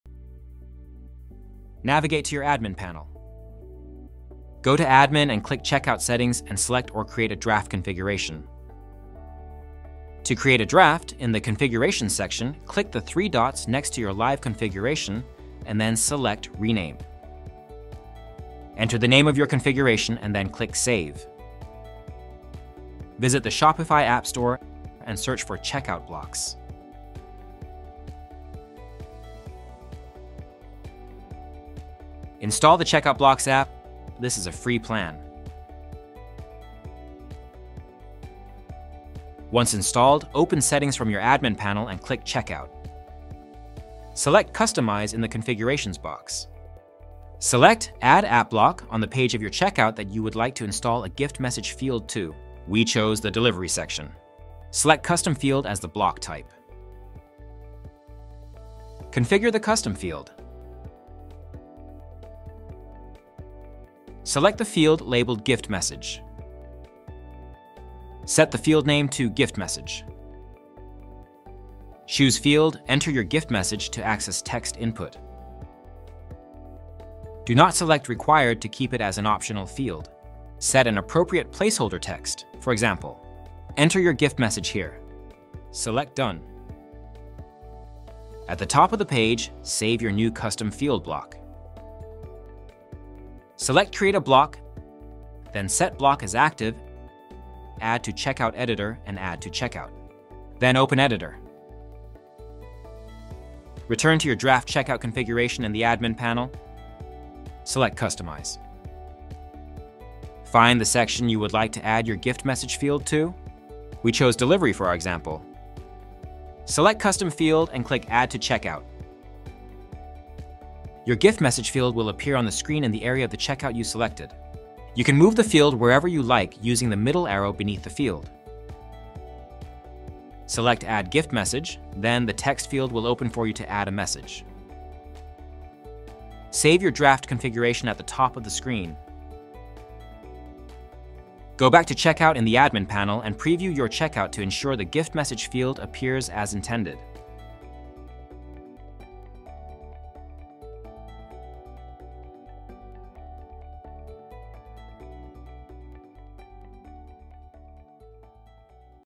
Adding a Custom Field with Checkout Blocks|| Shopify Academy
Add a special gift message to your checkout in minutes with Checkout Blocks!

Learn how to add a gift message option to your checkout process. This simple addition can increase customer satisfaction, reduce support inquiries, and create a more personalized shopping experience for gift-givers.